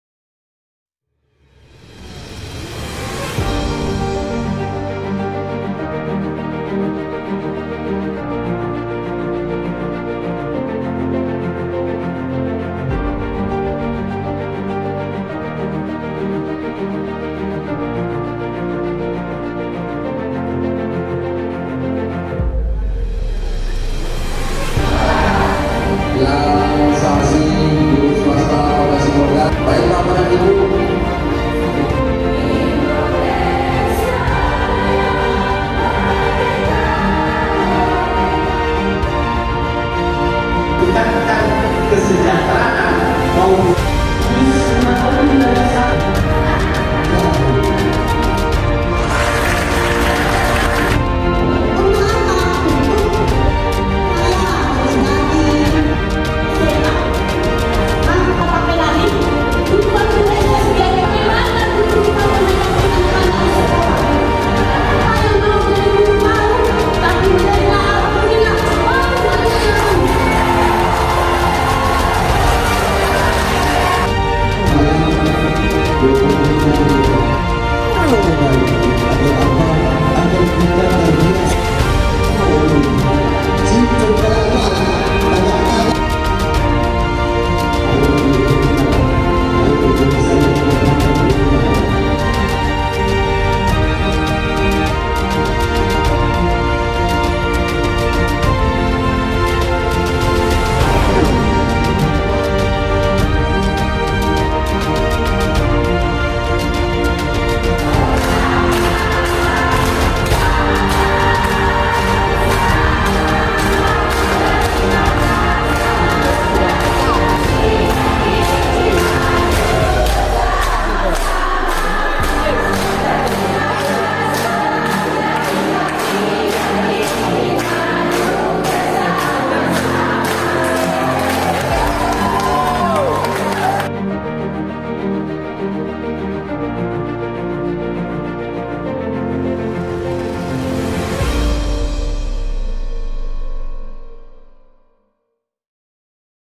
DISKUSI PANEL FORUM GURU SWASTA KOTA SIBOLGA UNTUK SERAP ASPIRASI GURU SWASTA
± 215 undangan guru2 Swasta dari berbagai sekolah
Hadir mengikuti diskusi panel serap aspirasi guru bersama ketua DPRD kota Sibolga,dengan harapan
Kedepannya agar lebih meningkatkan kesejahteraan para guru2 honorer dan swasta di kota Sibolga.